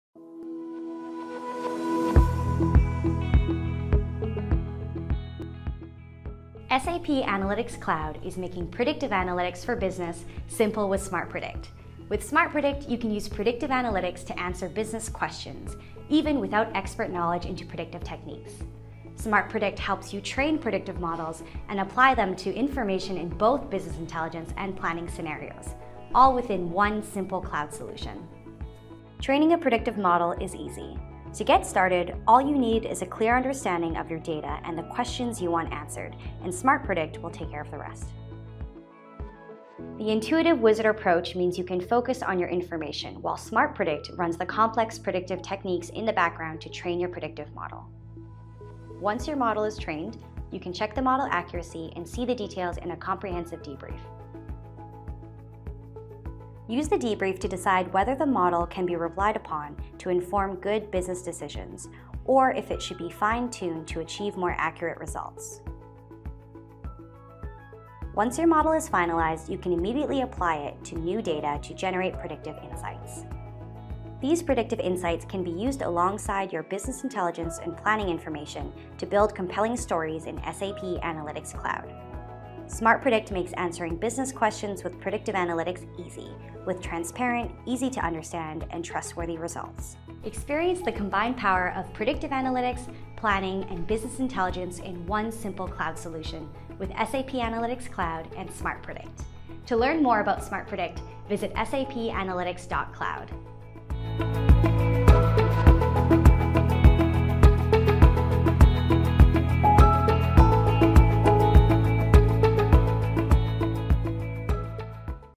SAP Analytics Cloud Feature Highlight: Smart Predict Overview
Predict future business outcomes and determine the best course of action with Smart Predict. This SAP Analytics Cloud feature helps business analysts answer questions about the future with predictive models created with machine learning technology--no data science experience required.